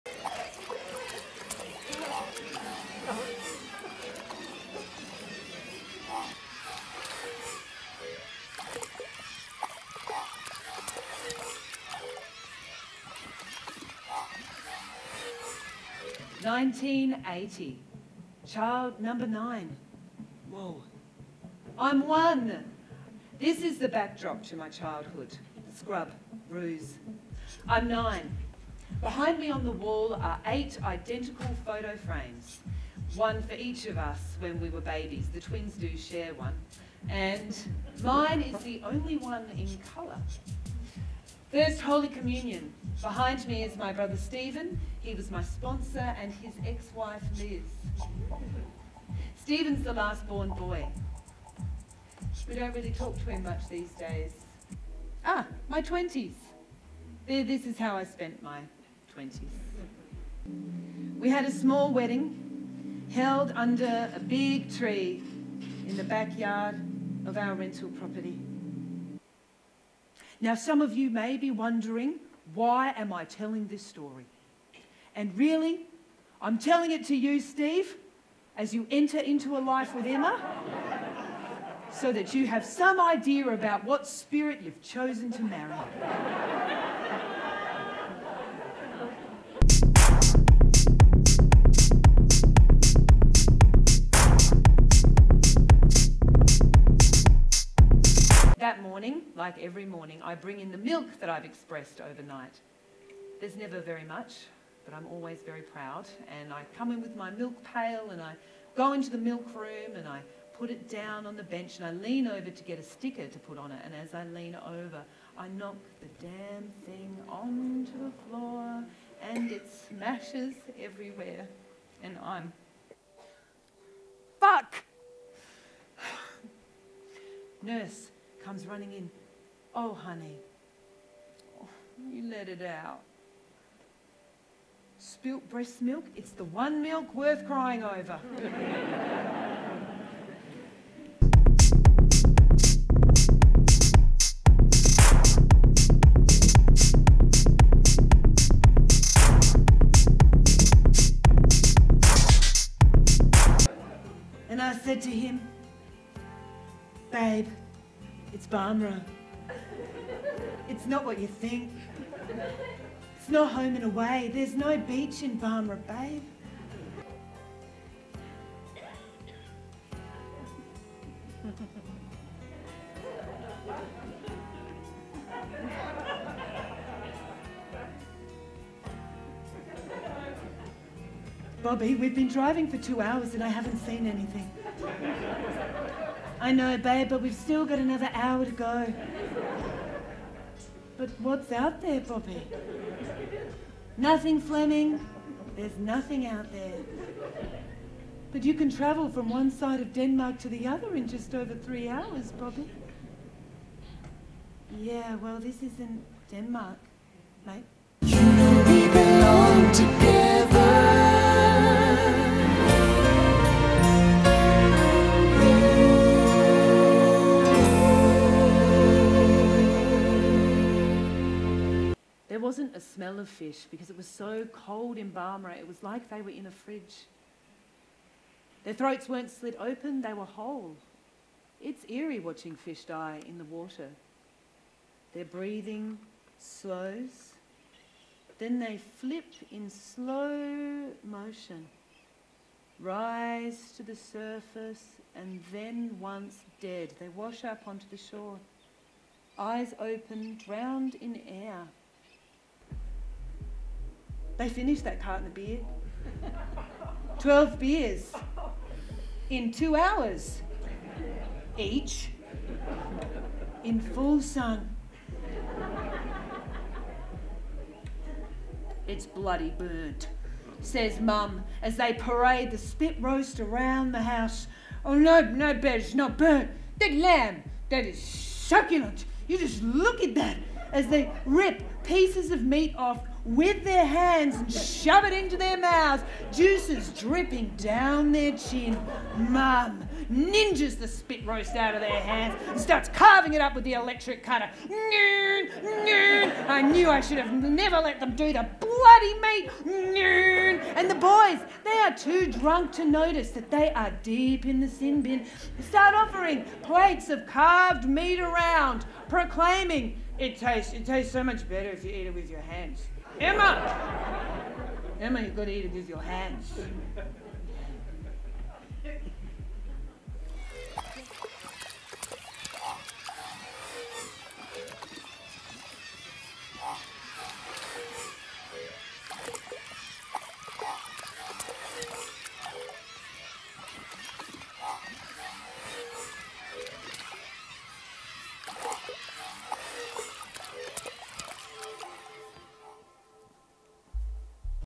The Photo Box by Emma Beech - Trailer
Emma Beech’s art is both simple and hard to define. Labels like ‘direct address performance’, ‘docu-drama’, ‘verbatim real-life portraiture’, all seem to rob her storytelling of its straightforward warmth and worth.

Conversations with other people have always been central to her shows, but a few years back something happened that persuaded her to turn the lens on herself. Her mum and dad gave her a box of family snaps and, as she had a performance that night, she thought it might be fun to rummage through them for the first time in front of an audience of strangers.

Amongst all the embarrassing hilarity of hairdos and frocks was a shot of her mum, taken aged 40, holding her baby self, the little surprise born 8 years after the last of her 8 (!) brothers and sisters. Emma, at the time, was 40 herself and mother of 5-year-old triplets. Out of that instant electric connection between two very different women 'The Photo Box' was born.

It’s about a town in regional South Australia where everyone knows you, a girl being left to make her own mistakes and grow herself up. Family, choices, memory, and mythmaking, and how the baby of a big Catholic brood became devoted to the search for unsentimental truths.

Borrowing from cinematic installation, The Photo Box is a funny and heartfelt new work from one of our most gifted performers.

Presented by Adelaide Festival, Vitalstatistix and Brink Productions.

This season of 'The Photo Box' ran March 3-7, 2022 at Space Theatre, Adelaide, Kaurna Yerta.

Video production: Replay Creative
Cameras: Heath Britton and Edwin Kemp Attrill
Editing: Heath Britton and Jennifer Greer Holmes